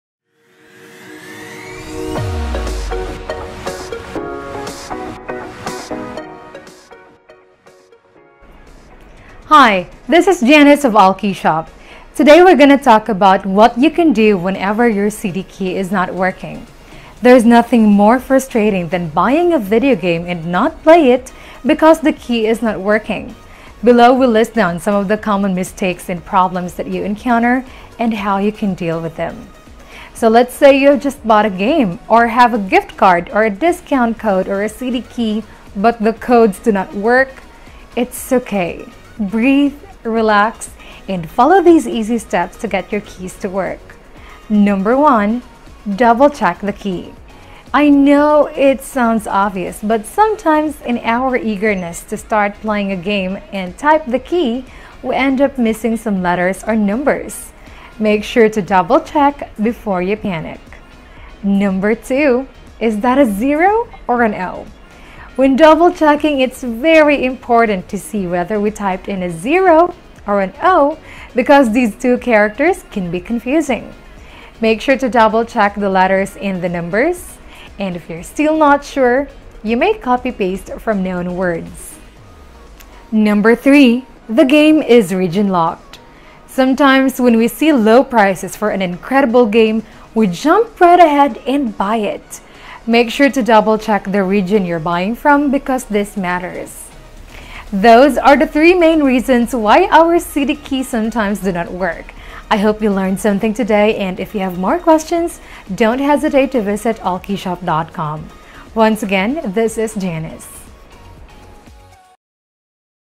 What can i do if my CD Key is not working | How to fix for all platforms
The complete tutorial for all platforms to make your CD Key working.
Fix the most common issues with this Allkeyshop guide.

If your problem persists, just contact our support team (Green Help Button down right on the website)